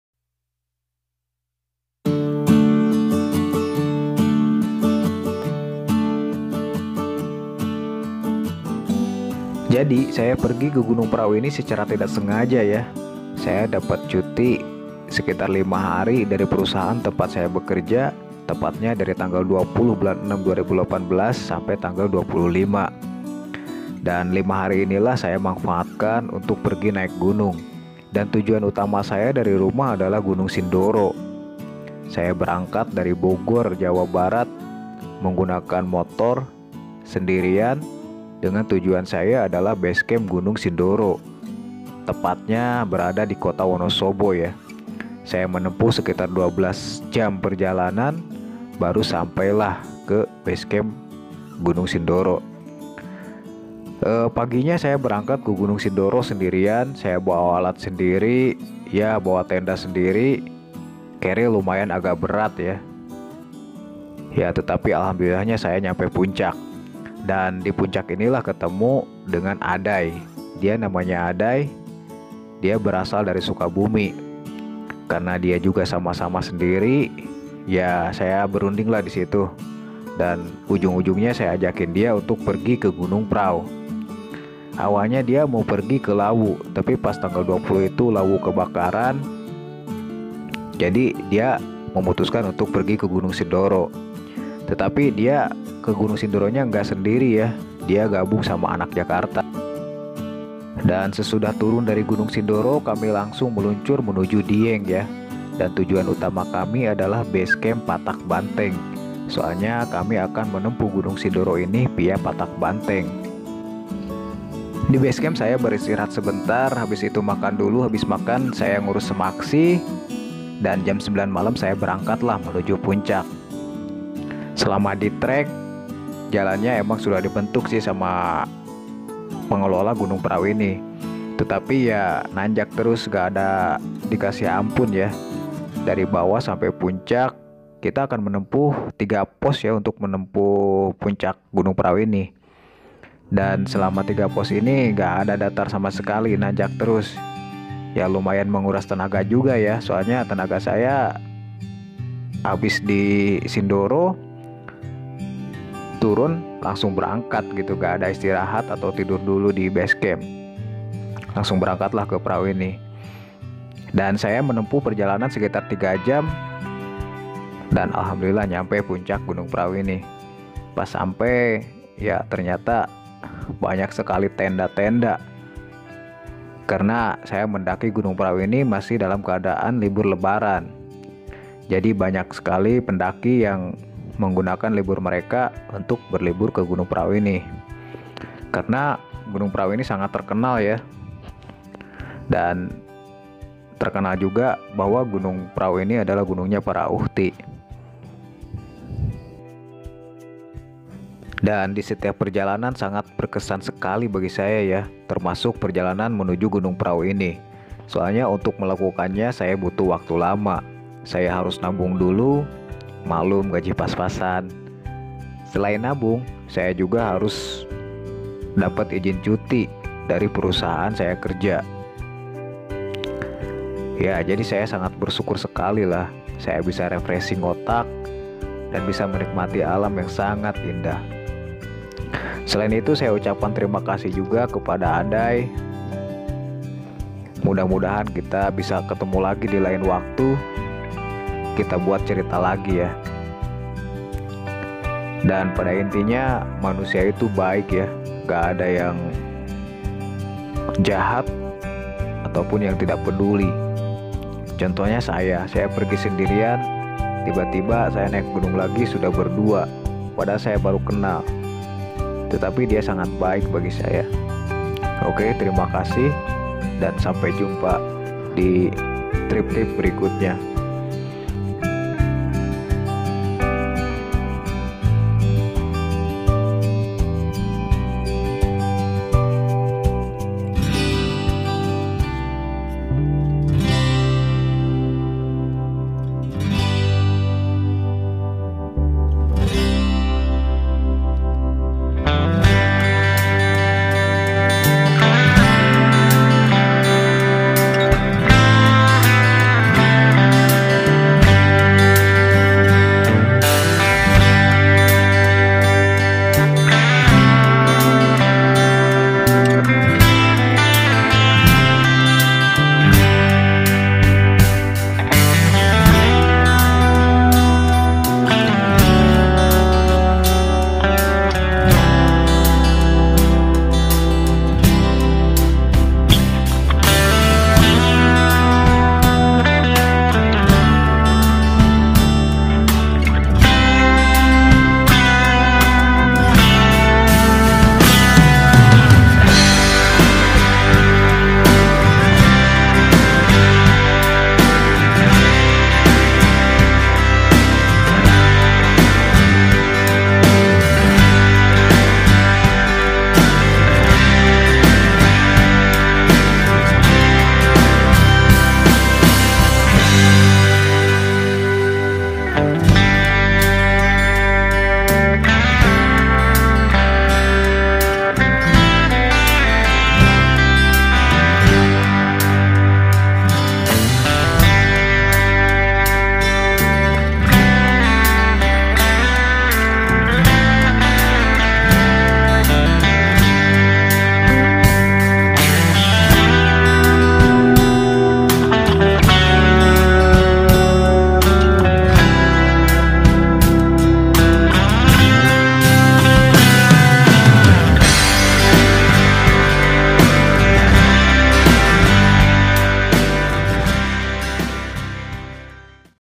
GUNUNG PRAU VIA PATAK BANTENG
Pemdakian gunung parau via dieng wonosobo
Jalur patak banteng
Modal nekad...low budget .nekad bener nekad